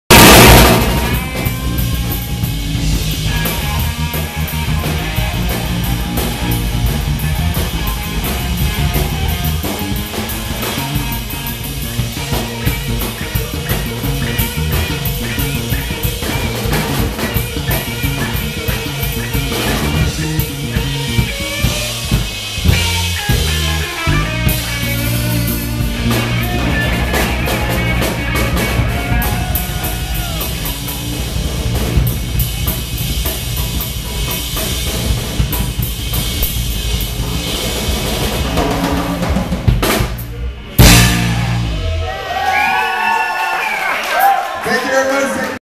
Derek Falls Trio at La Chapelle (final few seconds of the evening)
With:  Derek Falls (vocals/guitar), Gary Mckenzie (bass), Nasyr Al-Khabyyr (drums)